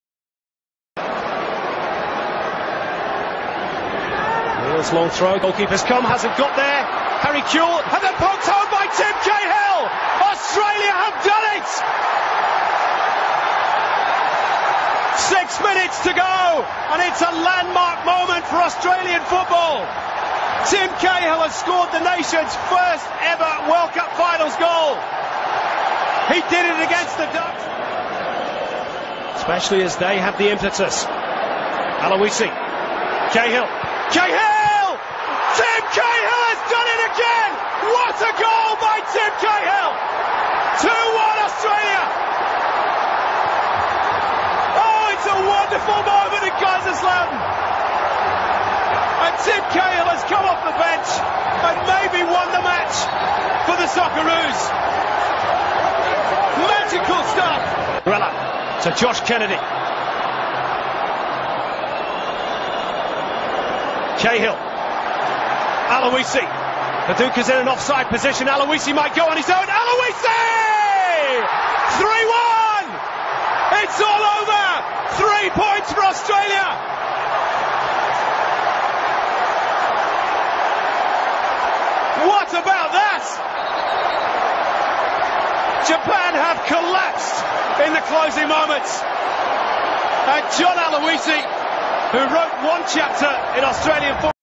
BEFRESHSPORTS - Australia First World Cup Win
Classic Australian Moment

No one thought we could beat Japan who was a powerhouse in Asian football but our Socceroos showed the fighting spirit and the world how good we were on the world stage.

Socceroos moment